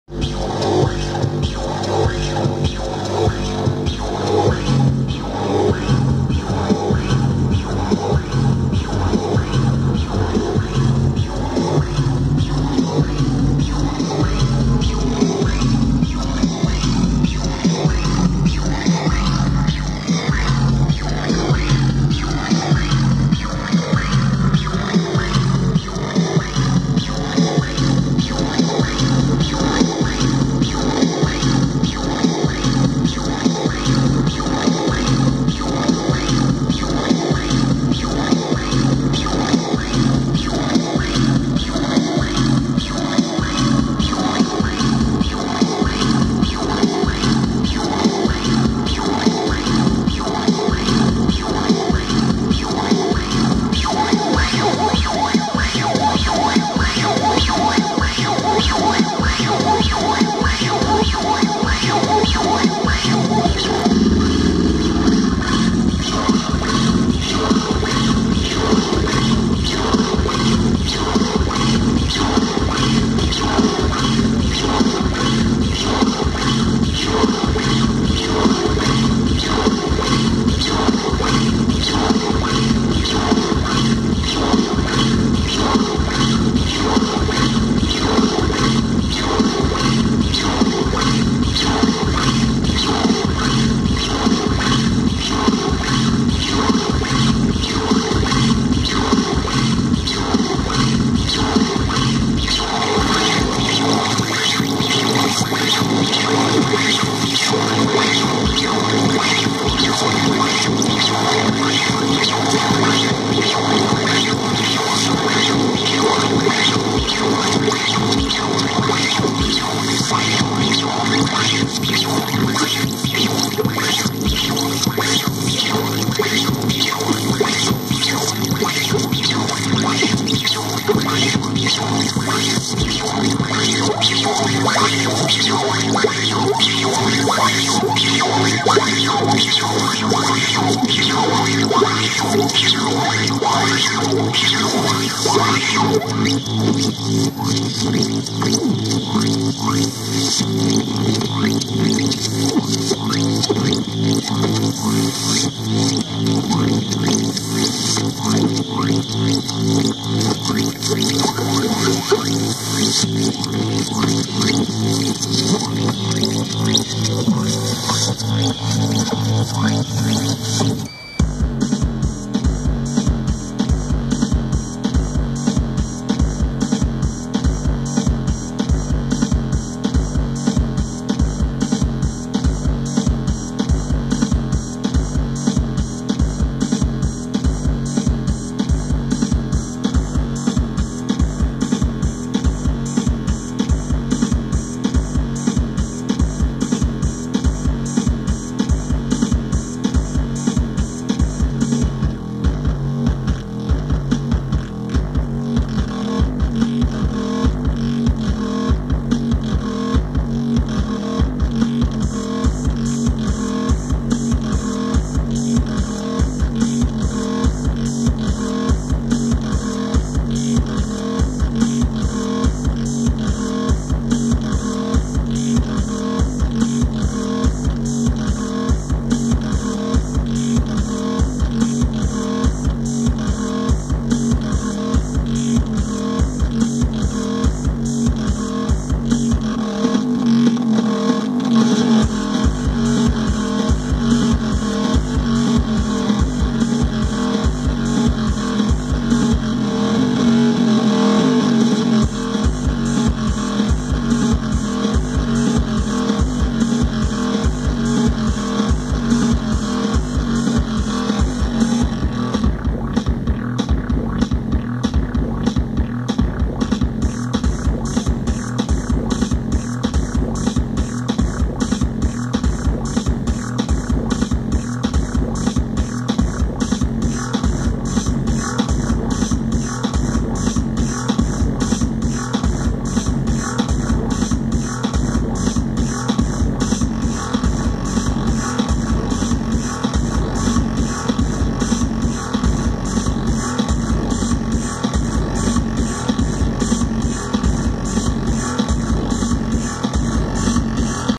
Container Sept 8 2016
More fragments from Burnside Park, Providence RI. "International technological artist" Ren Schofield.